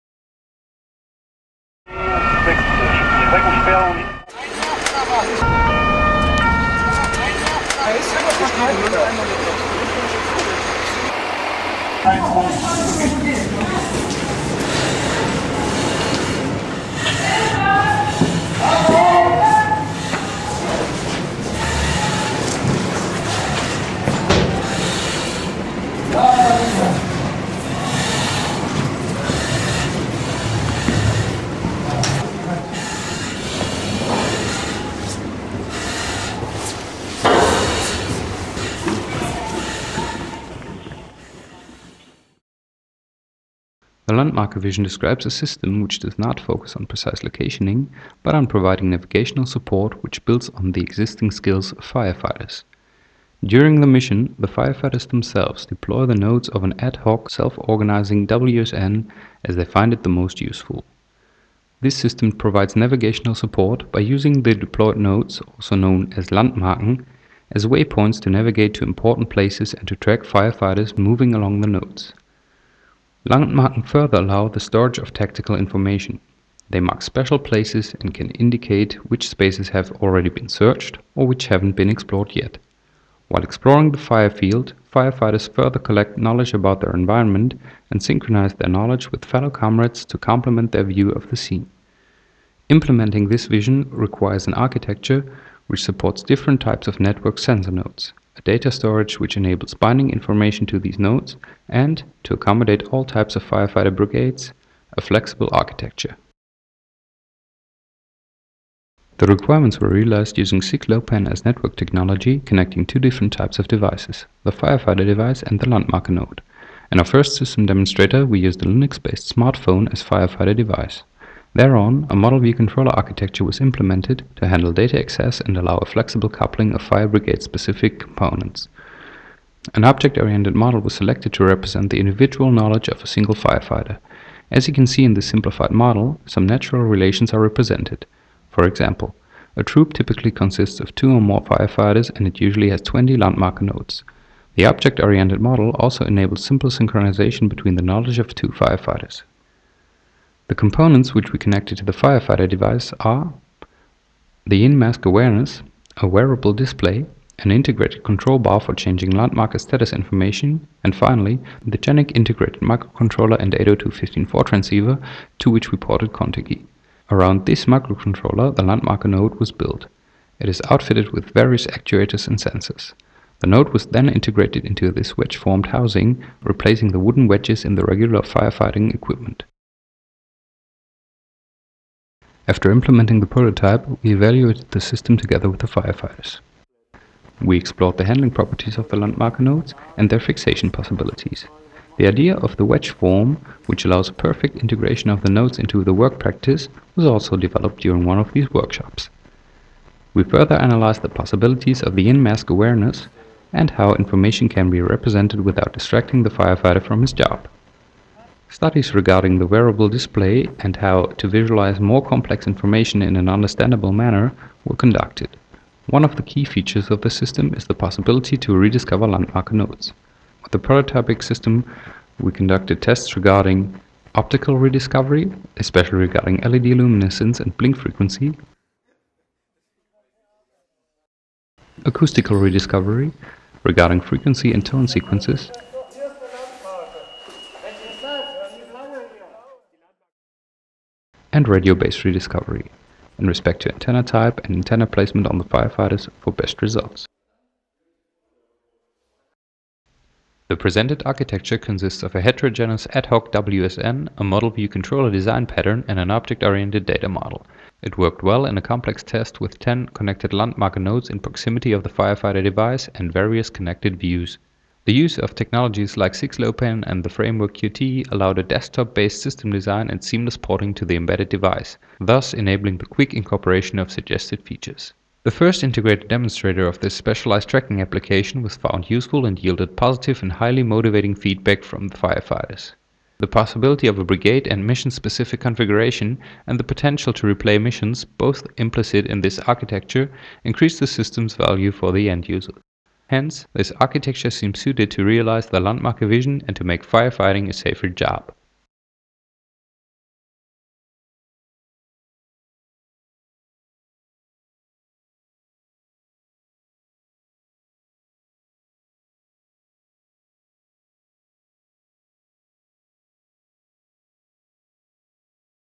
A MVC prototype for the landmarke firefighter navigation system
This video shows a first complex system demonstrator of the "landmarke" system which was developed at the Chair for Pervasive Computing Systems / TecO of the Karlsruhe Institute of Technology (KIT), Germany.

Landmarke describes the vision of an ad-hoc WSN which is created by the firefighters during a mission to augment the environment in order to provide orientation in harsh indoor environments.

The video was first published in November 2010 at the Internet of Things Conference (IOT 2010), Tokyo, Japan.